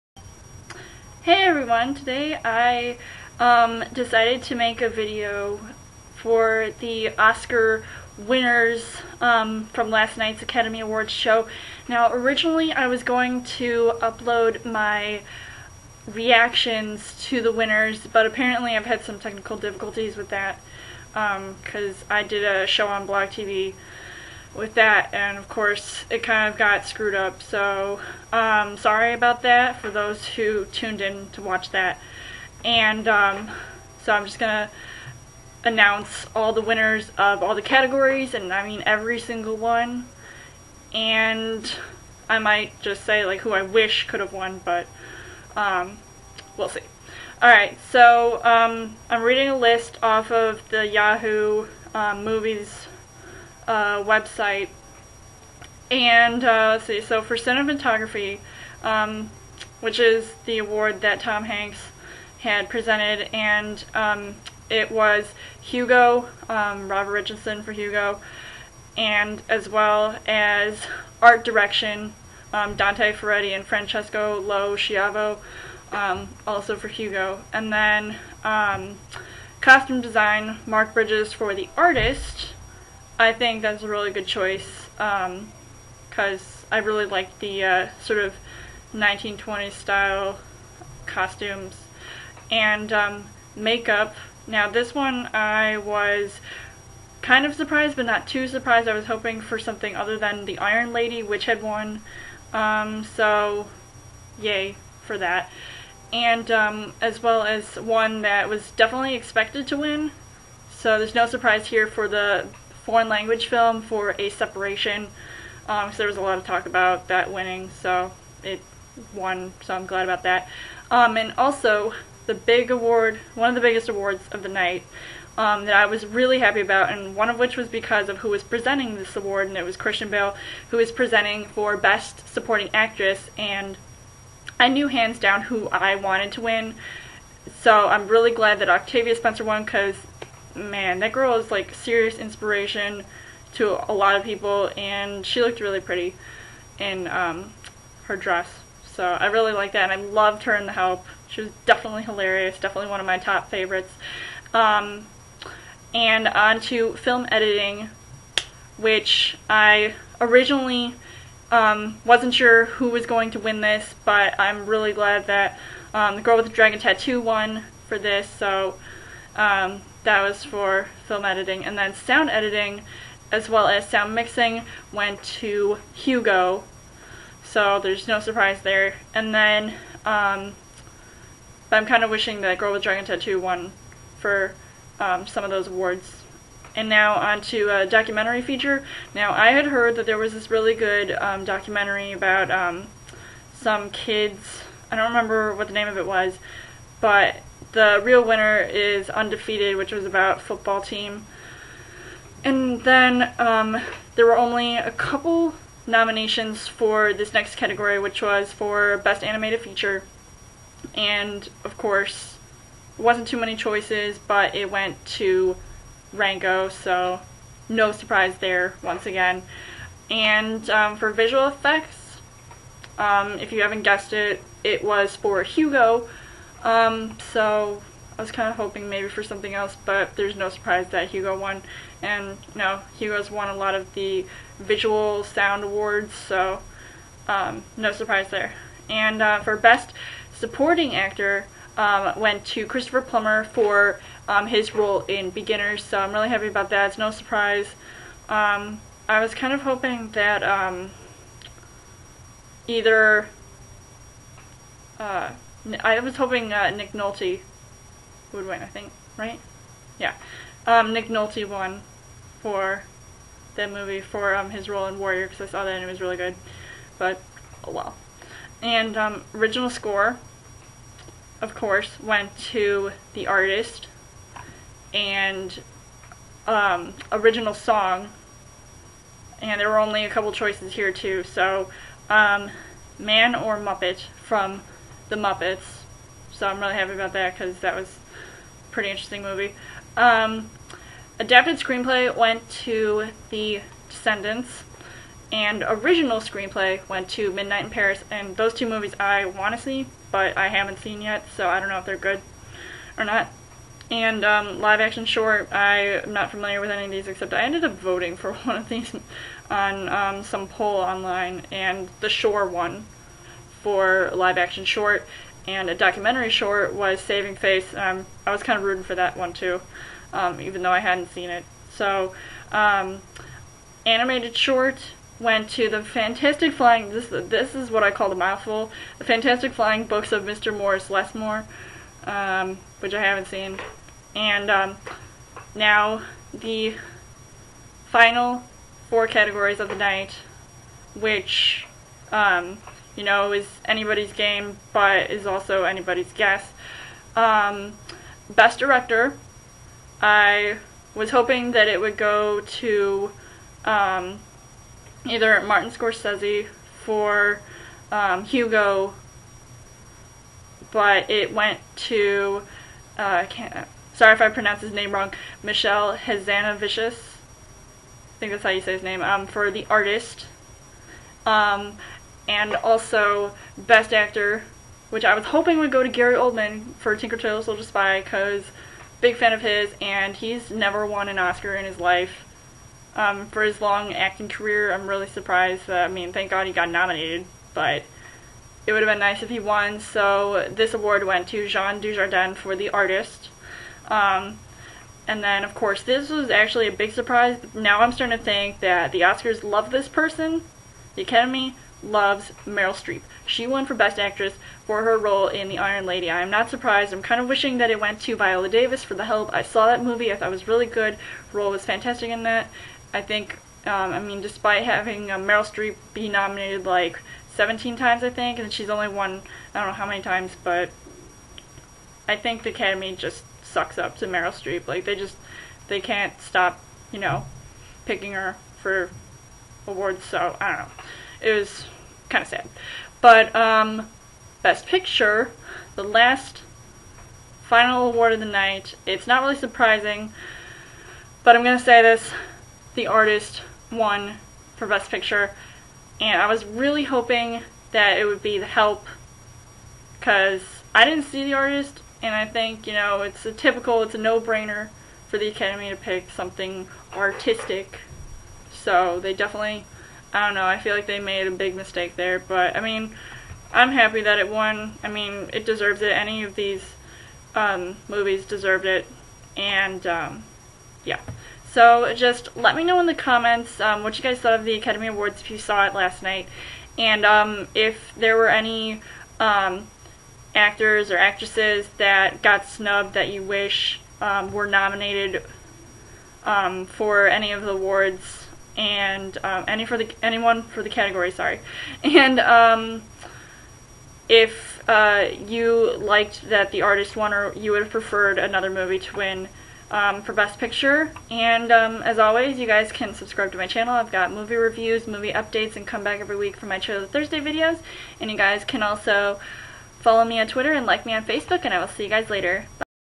Oscars 2012 Winners Review
And what movie did you want to see take home the Best Picture award?

Who was the best dressed?  Which acceptance speech did you like?

What did you think of Billy Crystal as a host?